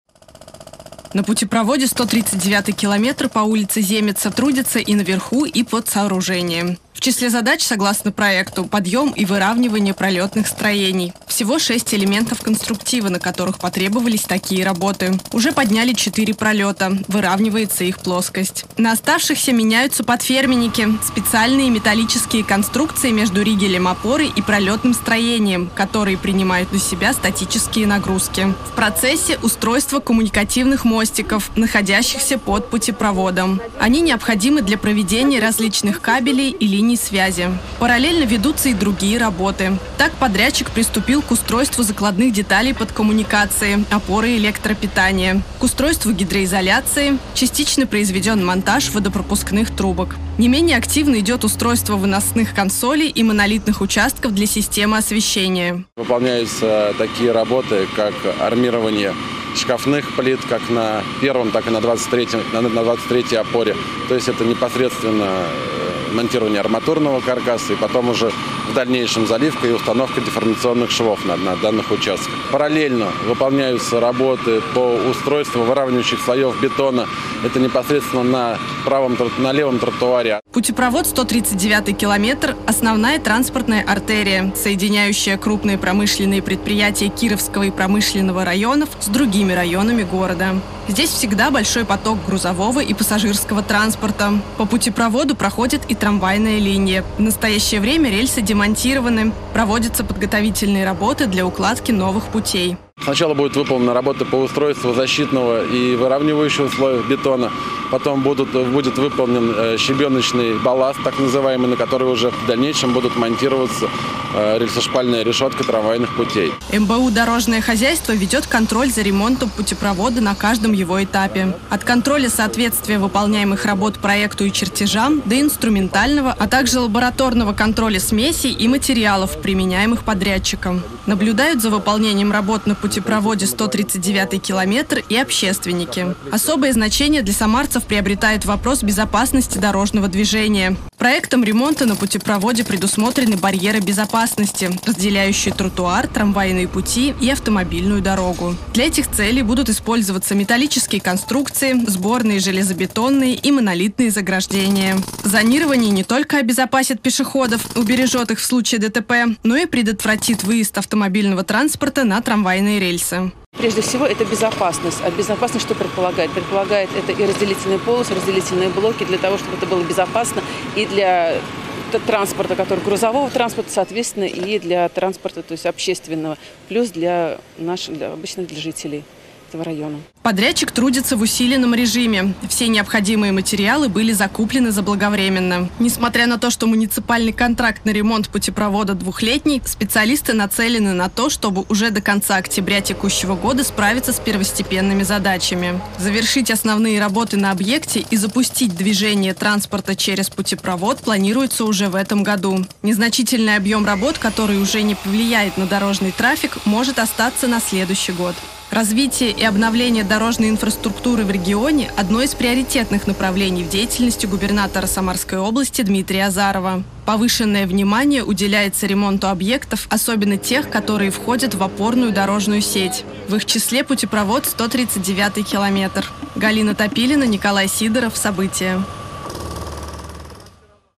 В Самаре уже в основной фазе работы по обновлению путепровода "139 километр"
В Самаре в активной фазе – работы по обновлению путепровода "139 километр", проходящего через улицу Земеца. Они выполняются на средства областного и городского бюджетов. Ремонт стартовал 10 апреля, с тех пор действует временное ограничение движения транспорта по улице Земеца - от въезда на территорию завода  "РКЦ "Прогресс" до улицы Литвинова. На сегодняшний день основной демонтаж на объекте завершен, специалисты выполняют строительно-монтажные работы. За ними следят общественники, а сегодня наблюдала и наша съемочная группа.